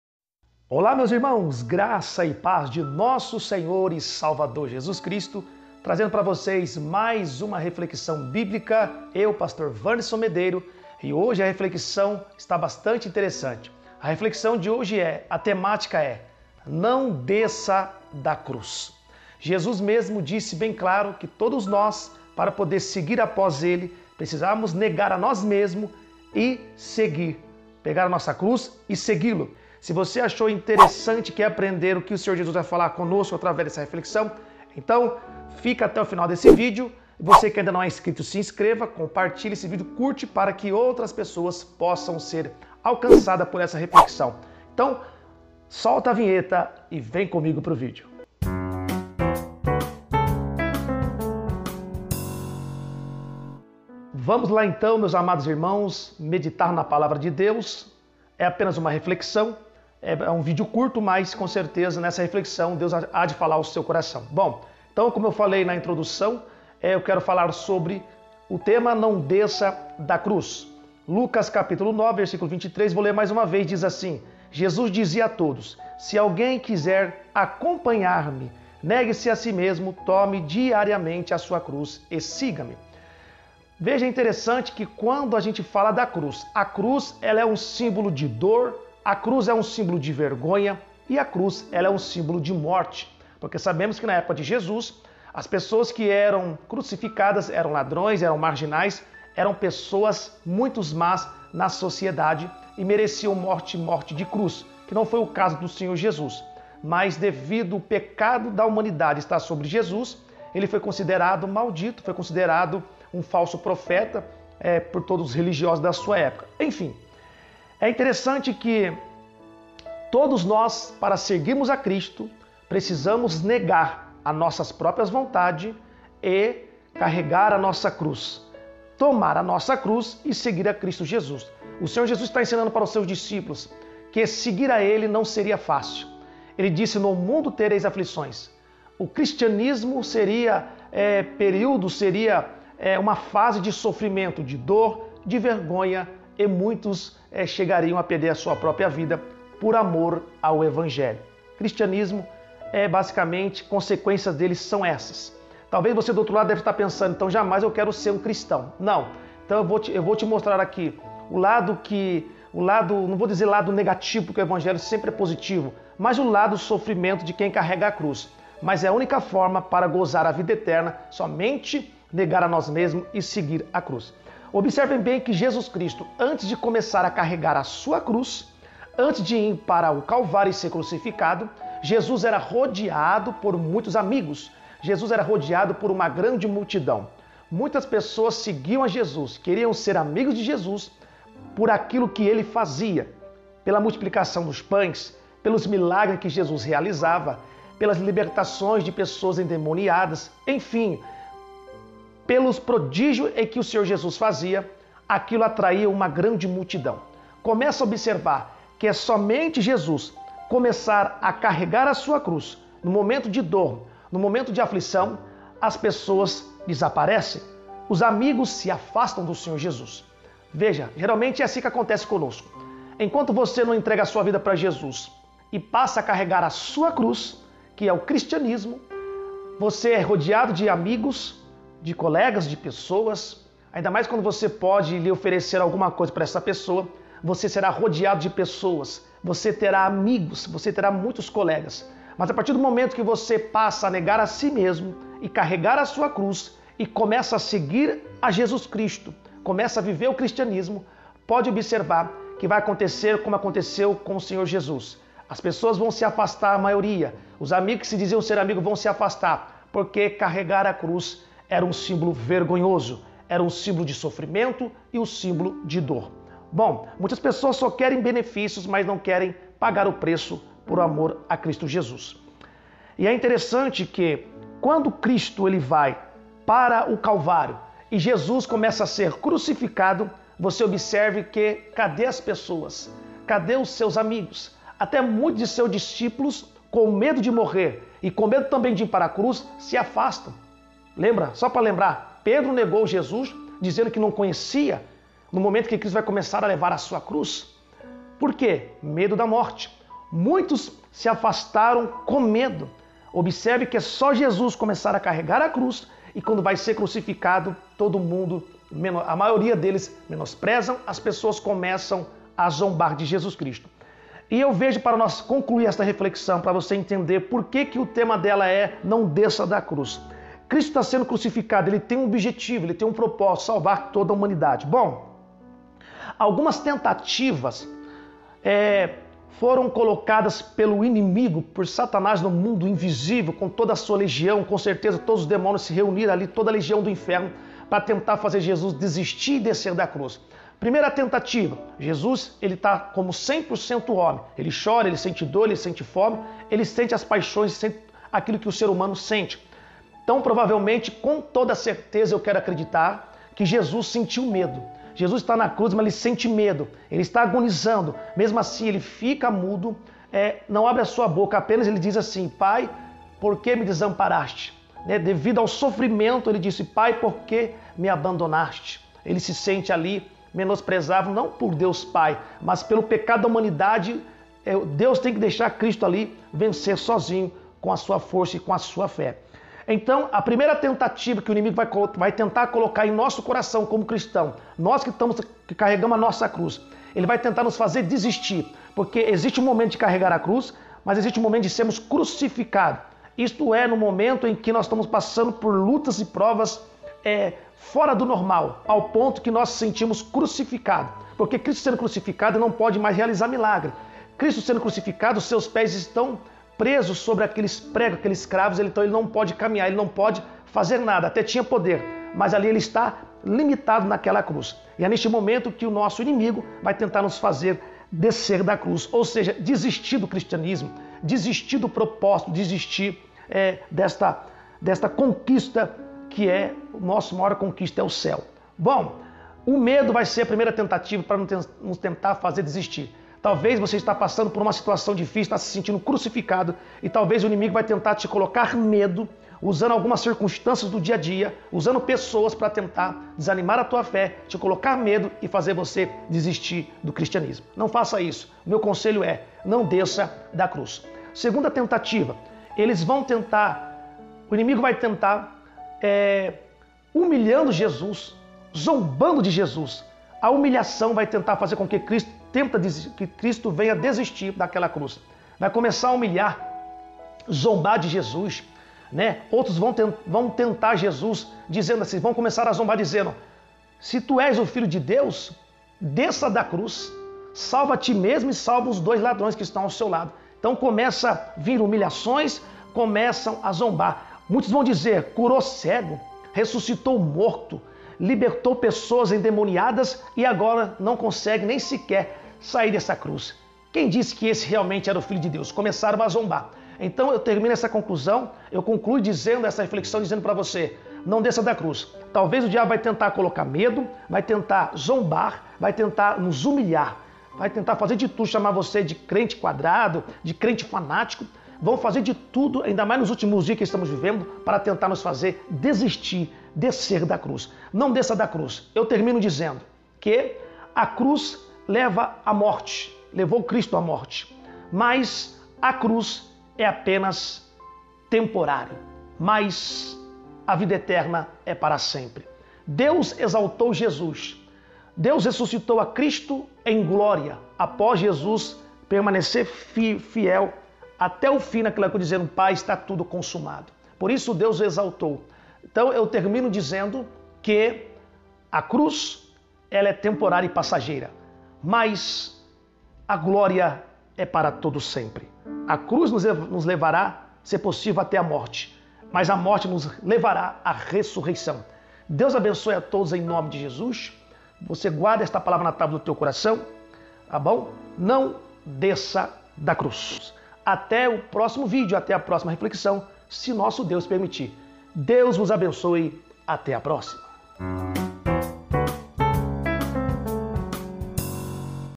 ✅Não desça da Cruz/Reflexões Bíblicas/Wanderson Medeiros
✅Não desça da Cruz
Jesus disse quem quiser seguir após ele, deve negar a si mesmo e carregar sua cruz.
Todo cristão tem uma cruz para carregar, e sempre nosso inimigo vai tentar de todas as formas, para nos fazer desistir de carregarmos a cruz.
Existe momentos em nossas vidas, que vamos enfrentar situações tão difíceis, que vamos nos sentirmos crucificados e é nesse momento que, nosso inimigo vai nos tentar nos fazer descer da cruz.
Algumas tentativas aconteceram para que Jesus descesse da cruz, desistisse do seu propósitos.
Mas Jesus foi fiel até a morte, mas Deus o ressuscitou em Glória, assim será para quem carregar a cruz até o fim.
O diabo sempre irá tentar nos fazer descer da cruz, mas se mantermos firmes carregando nossa cruz até o fim, vamos receber a vida eterna!
  Assistem outros vídeos nos links abaixo:
Meu Instagram
prwandersonmedeiros
Meu facebock